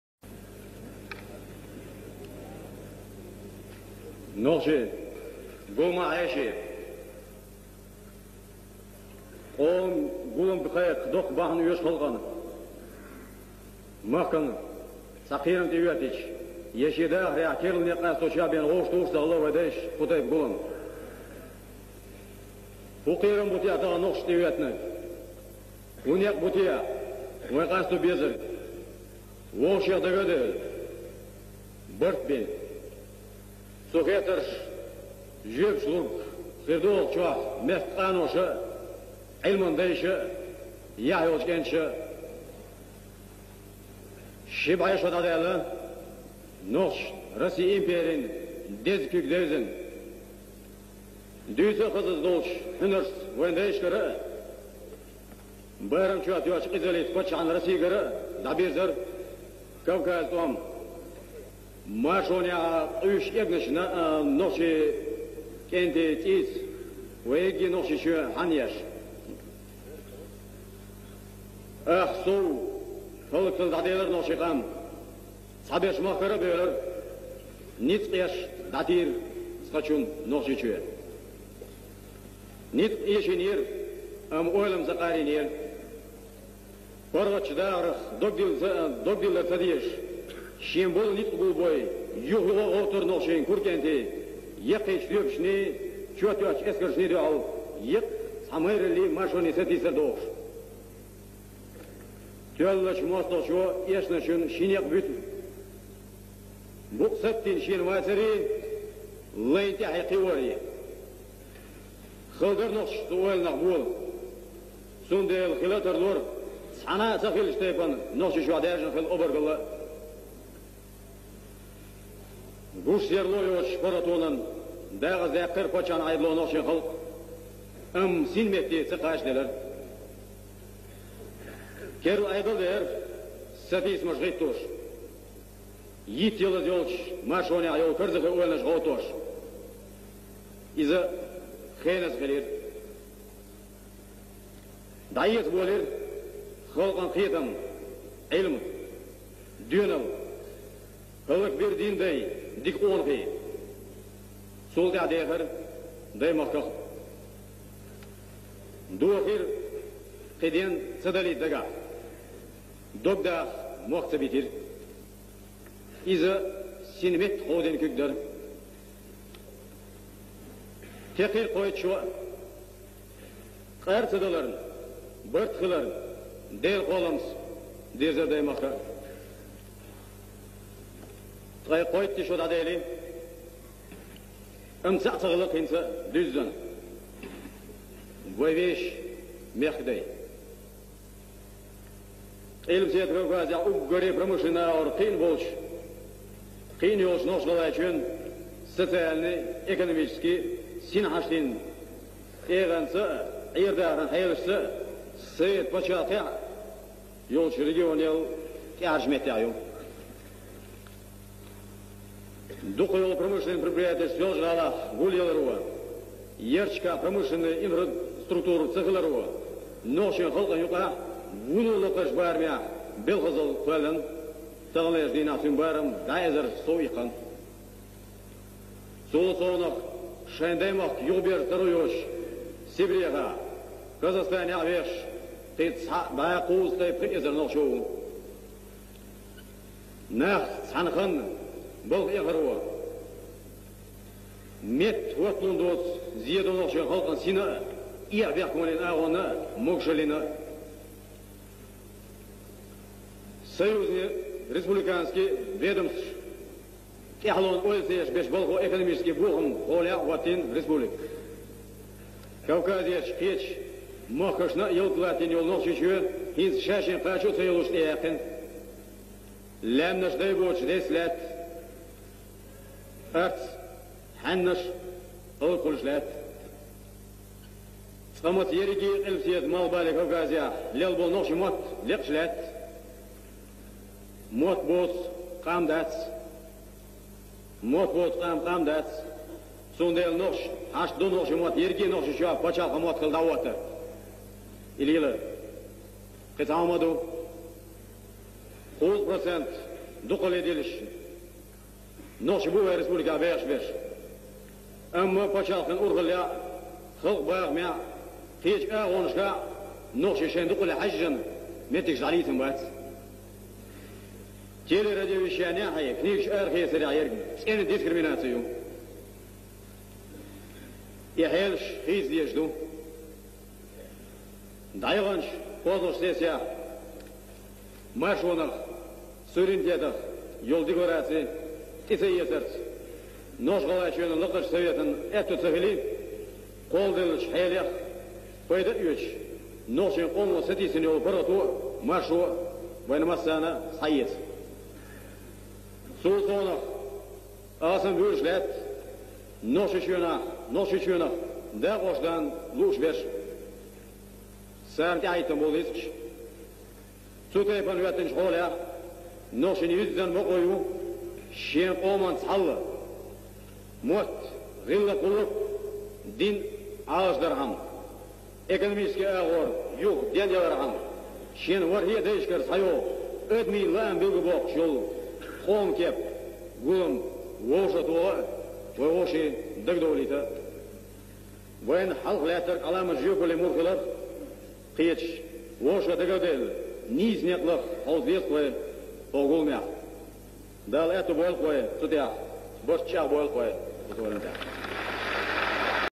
Умхаев Леча. Первый Общенациональный Съезд Чеченского Народа 23—25 ноября 1990 года.
Умхаев Леча - председатель Оргкомитета по созыву первого Общенационального Съезда Чеченского Народа, первый заместитель председателя ИК ОКЧН, народный депутат ЧИАССР.